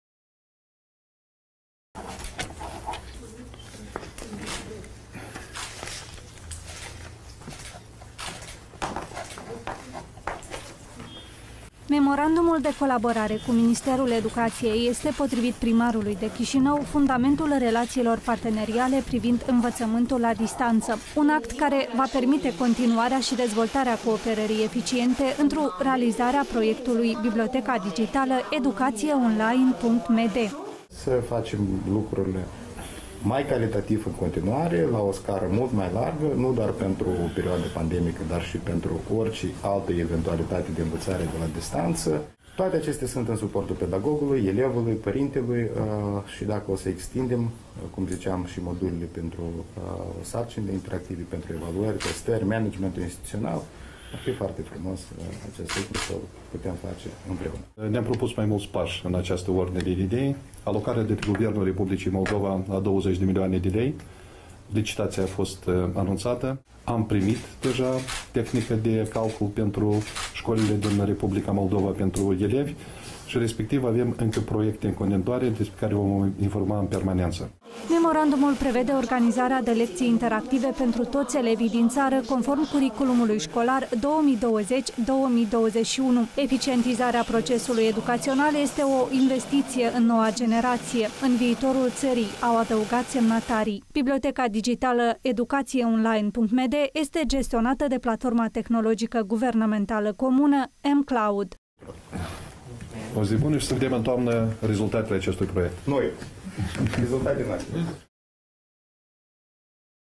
Lecții interactive pentru elevii din țară
Suport didactic pentru pedagogi, elevi, părinți, dar și identificarea metodelor interactive pentru evaluarea școlarilor sunt câteva prevederi ale  proiectului Biblioteca Digitală, implementat de Primăria Chișinău și Ministerul Educației. Șefii celor două instituții au semnat astăzi un memorandum de colaborare privind învățământul la distanță.

Memorandumul de colaborare cu Ministerul Educației este, potrivit primarului de Chișinău, fundamentul  relațiilor parteneriale privind învățământul la distanță. Un act care va permite continuarea şi dezvoltarea cooperării eficiente, în realizarea Proiectului Biblioteca digitală -„educațieonline.md”.

ION CEBAN, PRIMAR DE CHIȘINĂU
„Facem lucrurile mai calitativ, la o scară mult mai largă, nu doar pentru perioada pandemică, dar și pentru orice altă eventualitate de învățare la distanță. Toate acestea sunt în suportul pedagogului, elevului, părintelui și dacă o să extindem și modulele pentru sarcinile interactive pentru evaluări, testări, managementul instituțiional o să fie foarte frumos ca acest lucru să-l putem realiza împreună”.

IGOR ȘAROV, MINISTRUL EDUCAȚIEI,CULTURII ȘI CERCETĂRII
„ Ne-am propus mai mulți pași în această ordine de idei, alocarea de către Guvernul Republicii Moldova a 20 de milioane de lei. Licitația a fost anunțată. Am primit deja tehnica de calcul pentru școlile din Republica Moldova, pentru elevi și avem preconizate alte proiecte despre care vă vom informa”.

Memorandumul prevede organizarea de lecții interactive pentru toți elevii din țară, conform curriculumului școlar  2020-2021.
Eficientizarea procesului educațional este o investiție în noua generație, în viitorul țării, au adăugat semnatarii. Biblioteca digitală „educatieonline.md" este gestionată de platforma Tehnologică Guvernamentală Comună (MCloud).